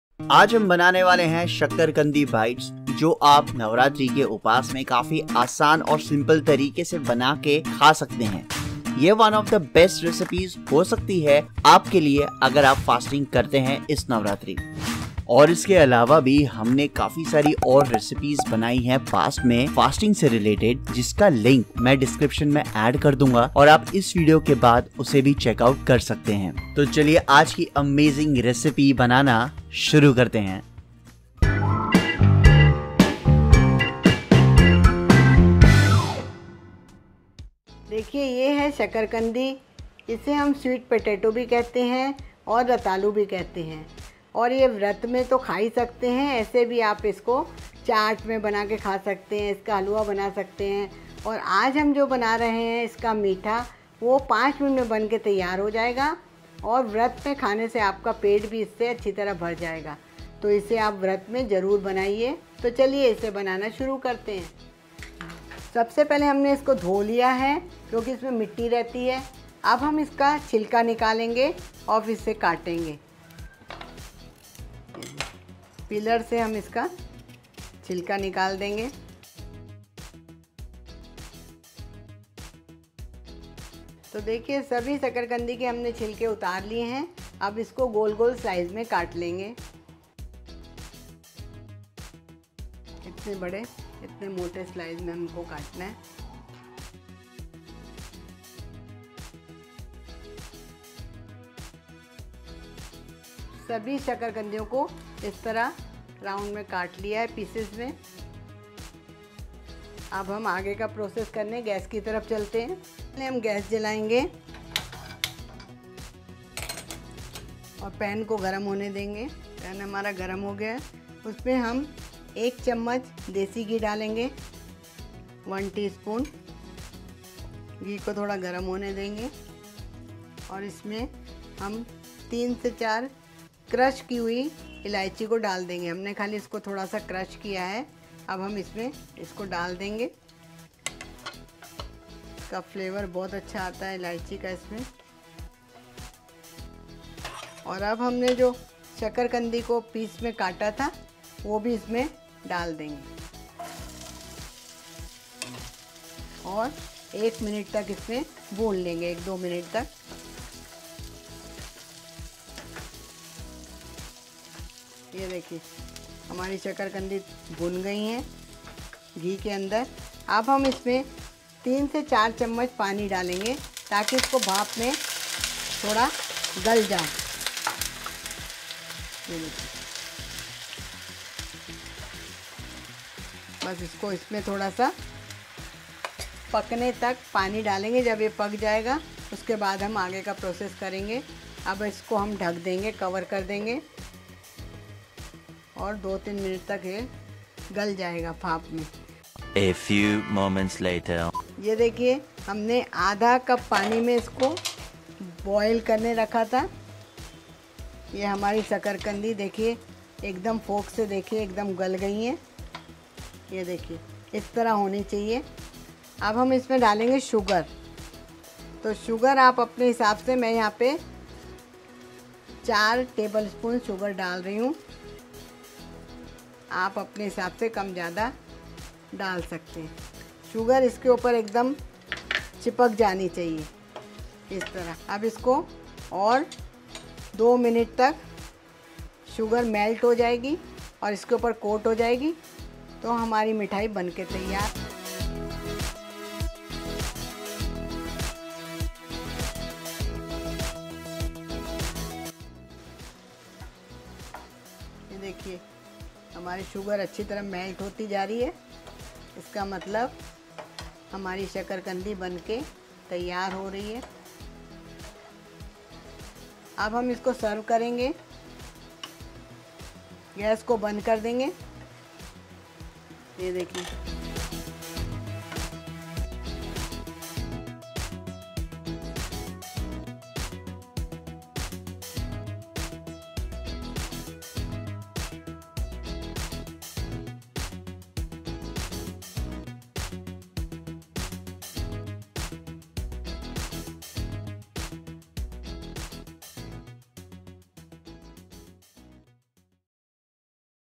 Shakarkandi Bites | Shakarkandi Recipe For Fasting|Sweet Potatoes | Upwas Recipe | Healthy Recipe
Shakarkandi or Sweet Potatoes is one of the most common dish and in this video, we have made Shakarkandi recipe for Vrat or Navratri Food for fasting
it's very easy to make so do watch our full video for Shakarkandi Bits for Navratri Food Fasting.

Query Resolved in this video
How to make Shakrandi
Shakrandi Recipe
Fasting Food Recipe